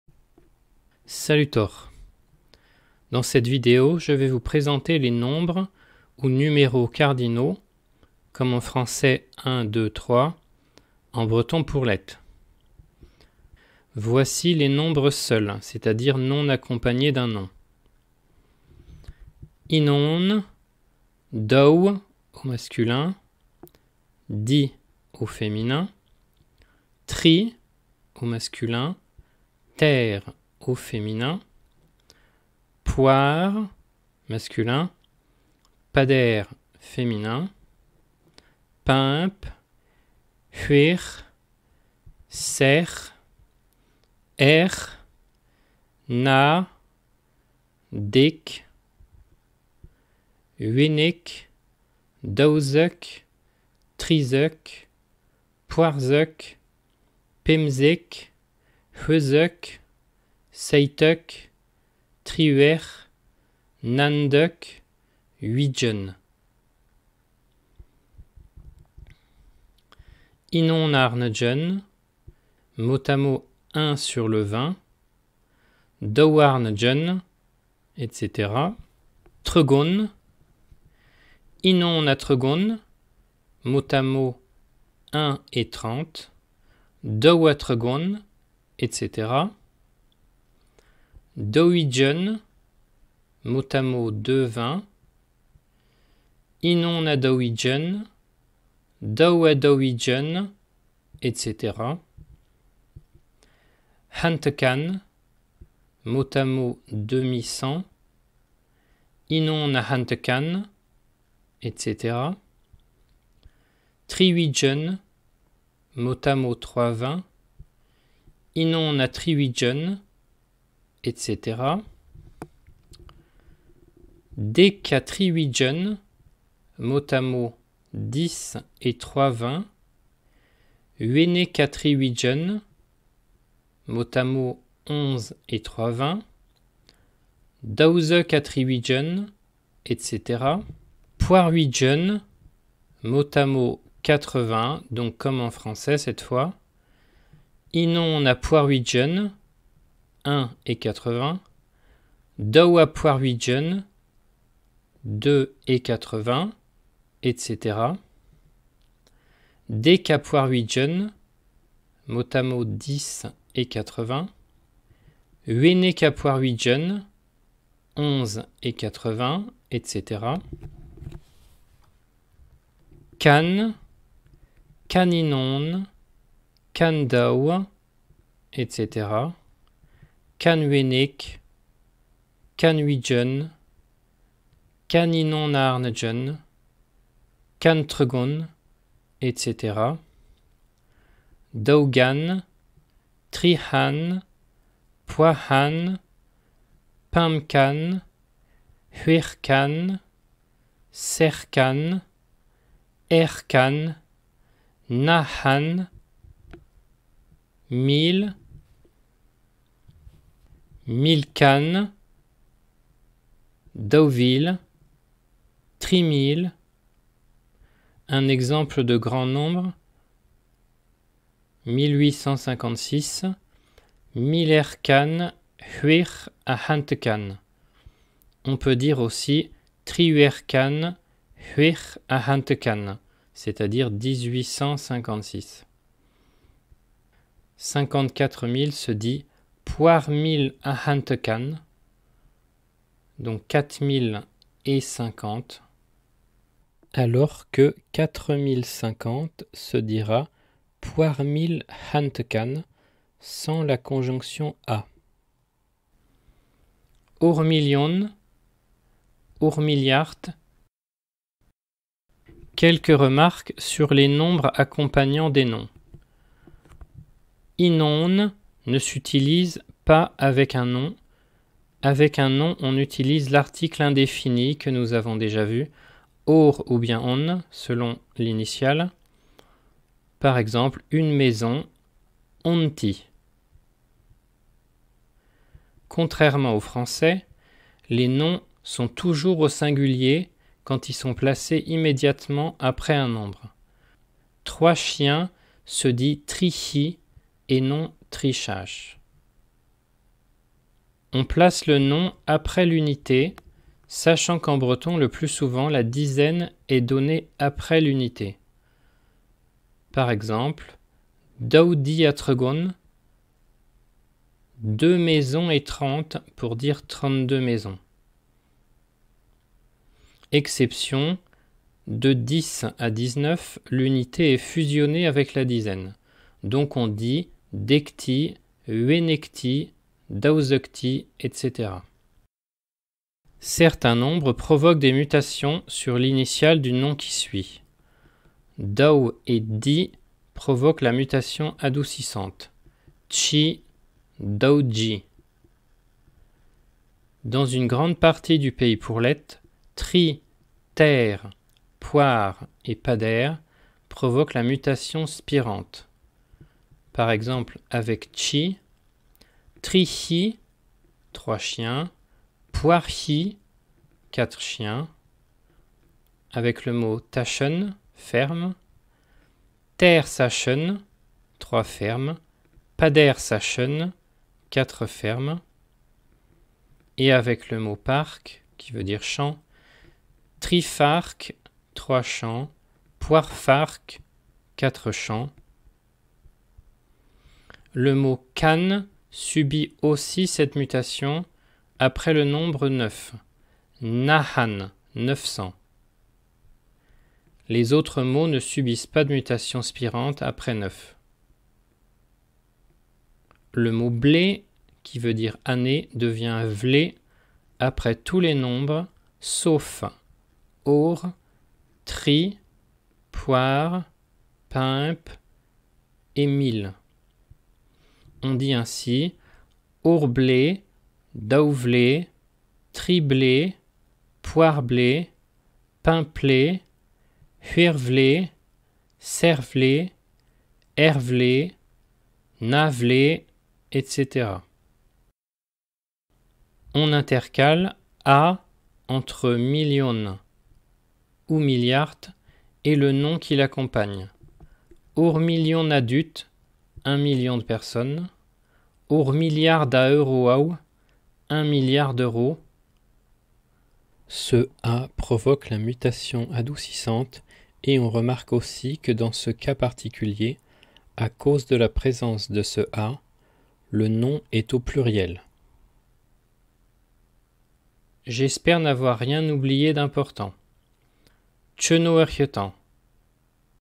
Les nombres (numéraux cardinaux : 1, 2, 3...) en breton vannetais pourlet
Dans cette vidéo, je vais vous présenter les nombres, ou numéraux cardinaux (français : un, deux, trois...) en breton pourlet.

Mersi braz da : / Remerciements à :
- tous les bretonnants de naissance sans qui je n'aurais pas appris à parler vannetais
- mes copains collecteurs
- Maïlys, de la chaîne pour les conseils de montage et les encouragements

Eid klewed tud ha 'gaoja breton a-vihan :/ Pour entendre des bretonnants de langue maternelle :